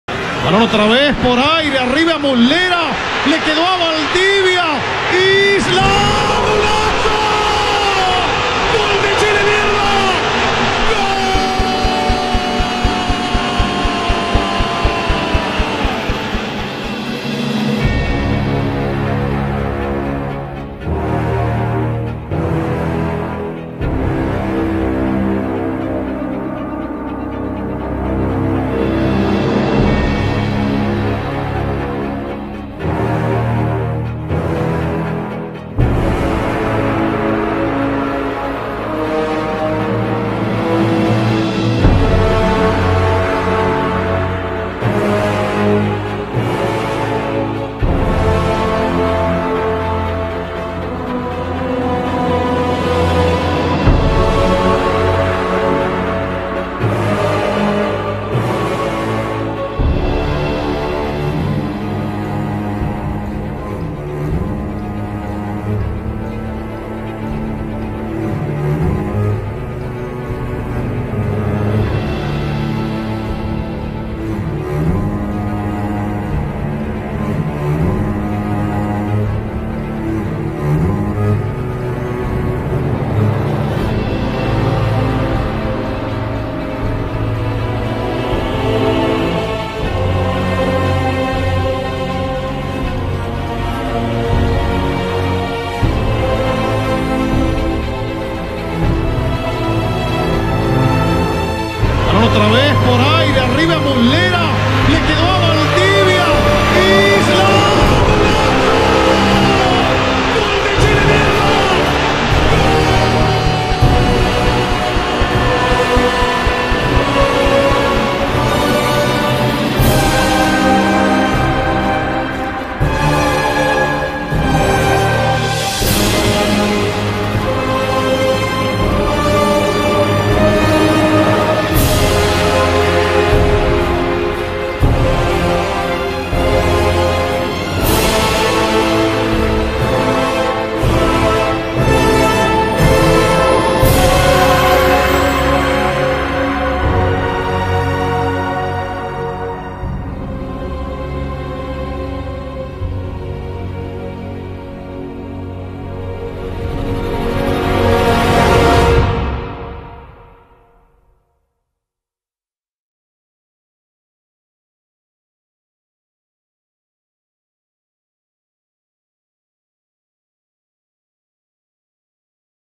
BEM-VINDO, Mauricio Isla! Jogador de raça, o novo lateral direito do Mengão
Faltam poucos detalhes! Mauricio Isla vem para substituir Rafinha, Isla é o lateral direito titular da seleção do Chile, e passou toda sua carreira na Europa, conquistando 5 títulos pela Juventus e sendo Bicampeão da Copa América com o Chile.

Vem, joga junto.

SRN!